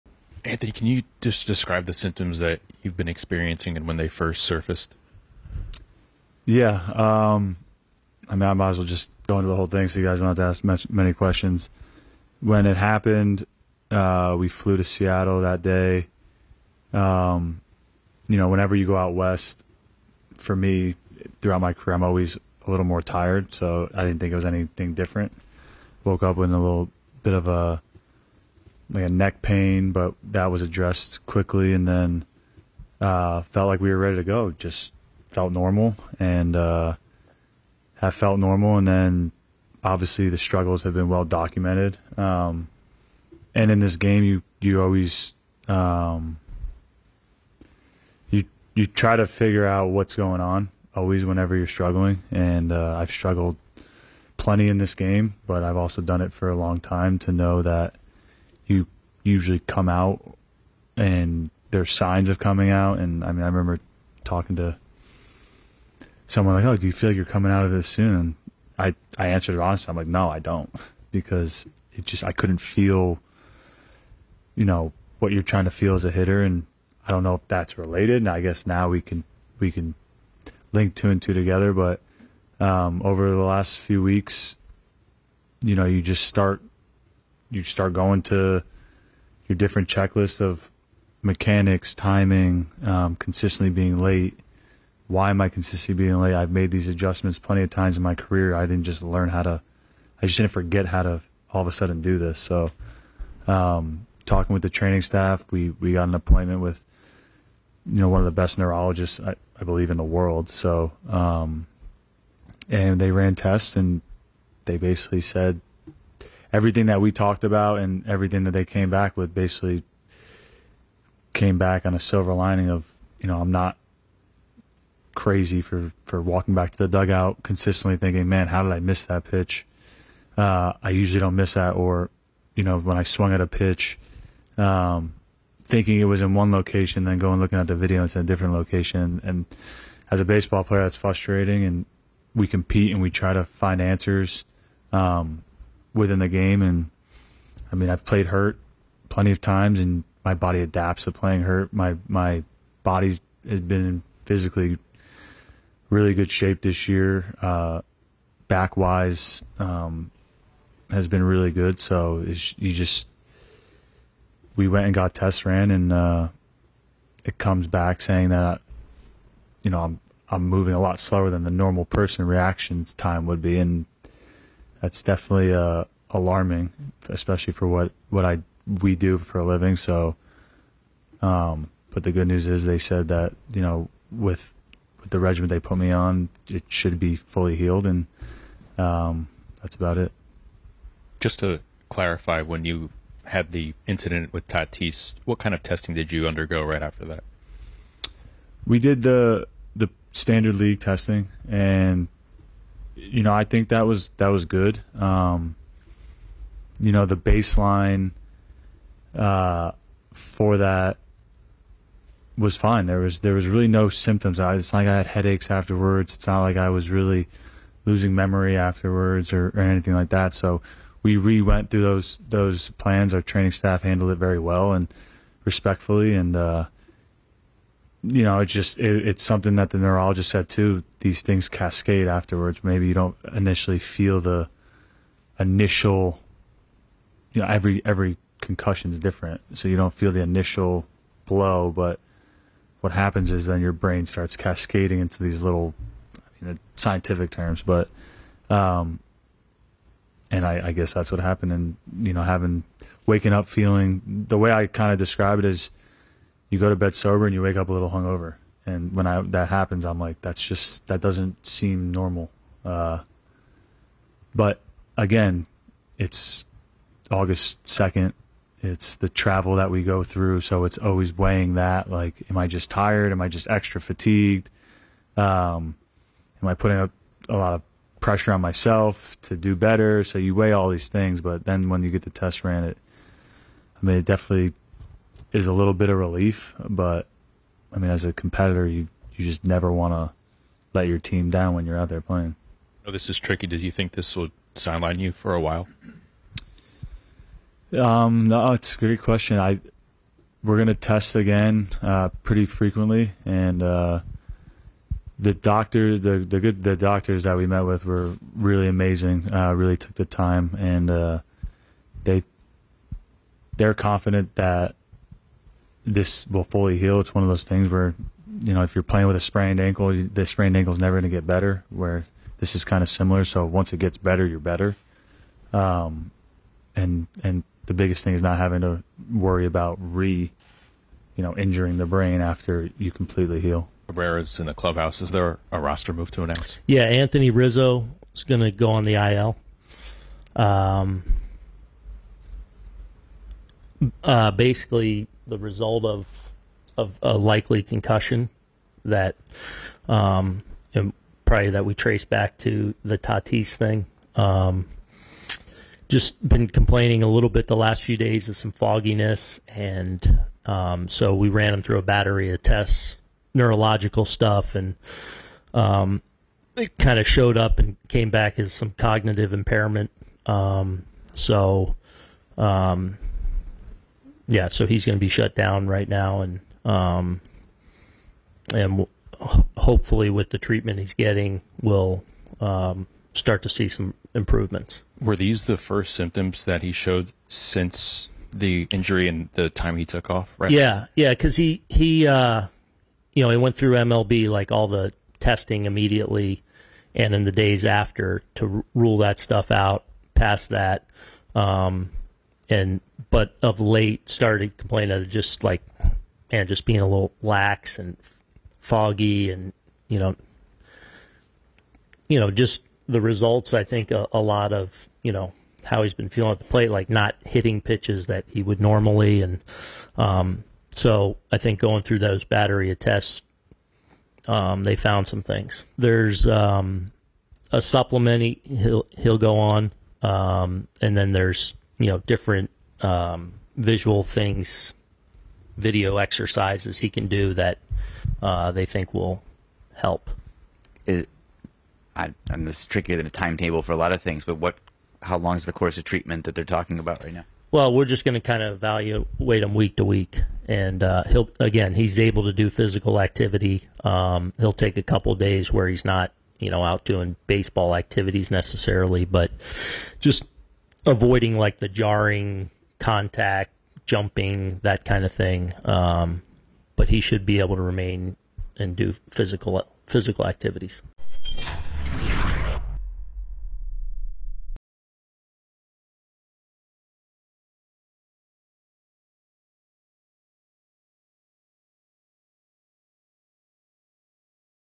Anthony Rizzo headed to IL with post-concussion syndrome in latest Yankees blow | NY Post Sports
Anthony Rizzo and Aaron Boone describe the symptoms and timeline that has led to Rizzo being placed on the IL with post-concussion syndrome from his collision with Fernando Tatis Jr. on May 28th.